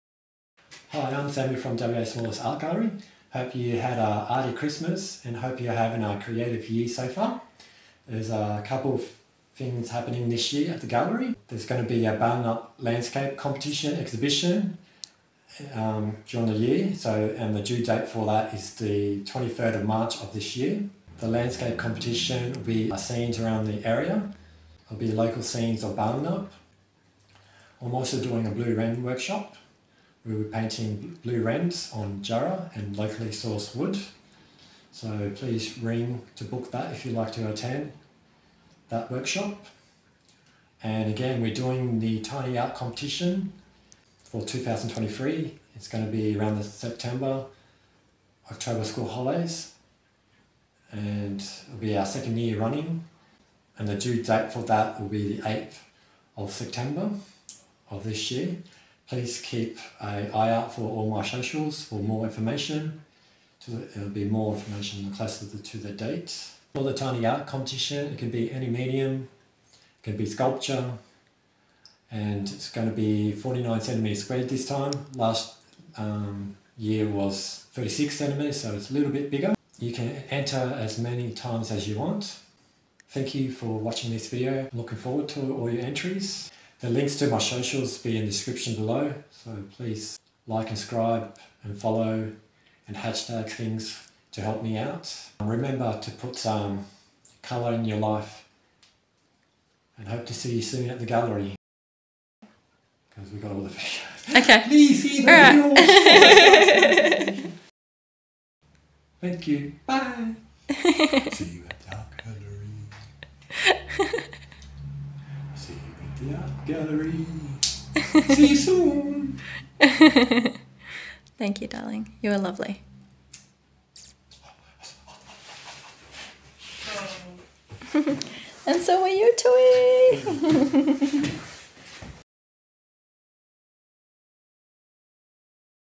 UPCOMING 2023 Art Competitions and Workshops
Competitions and Workshops held at WA's smallest Art Gallery in 2023...

COMPETITION:    Landscapes of Balingup
Due date:              23/03/2023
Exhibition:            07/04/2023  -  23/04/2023
Details:                  All mediums
Cost:                      $15 each

COMPETITION:    Tiny Art
Due date:              08/09/202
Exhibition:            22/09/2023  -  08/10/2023
Details:                  All mediums and subjects (G rated)
                               Size under 7cm cubed (49cm square)
                               Unlimited entries
Cost:                      $7 each

WORKSHOP:    Blue Wren on locally sourced wood
Date:                  21/06/2023
Cost:                  $48 (Coffee and cake included)